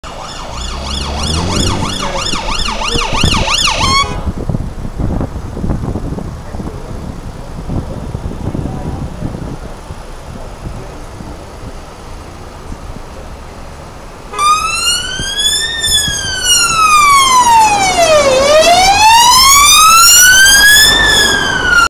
North Wales Police car on 999 emergency response Rhyl Sir Ddinbych/Denbighshire Cymru/Wales
North Wales Police car on 999 emergency response Rhyl Sir Ddinbych/Denbighshire Cymru/Wales, filmed in June 2016 via headcam